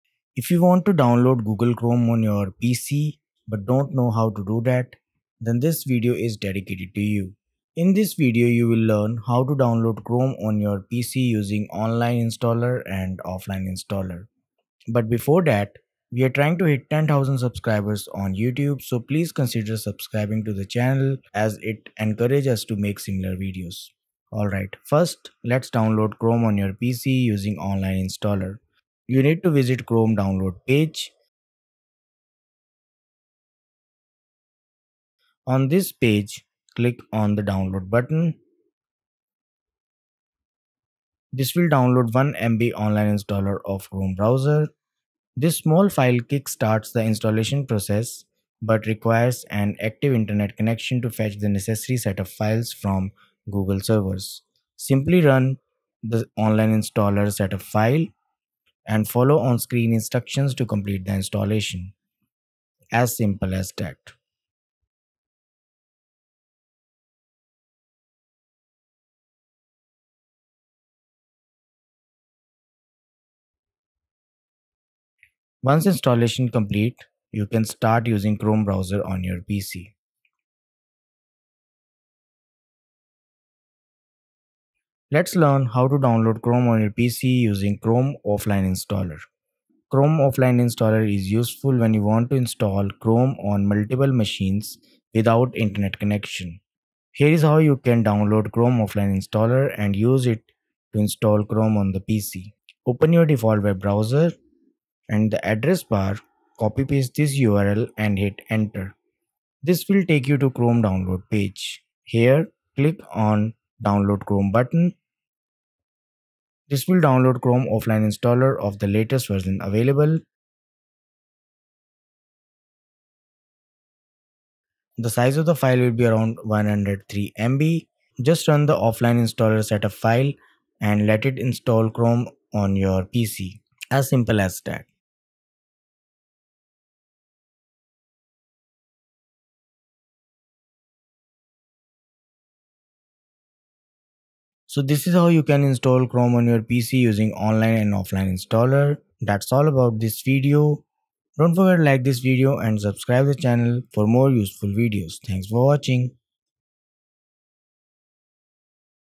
How to Download Google Chrome on PC (Windows 10 | Windows 11)
In this video, you will learn how to download Google Chrome on pc running Windows 11, Windows 10, and earlier versions of the Windows operating system

00:00 Introduction
00:25 Download Install Chrome using Online Installer
01:25 Download install Chrome using Offline Installer

FOLLOW @geekermag
FOLLOW @geekermag
FOLLOW @geekermag

RECOMMENDED DOWNLOADS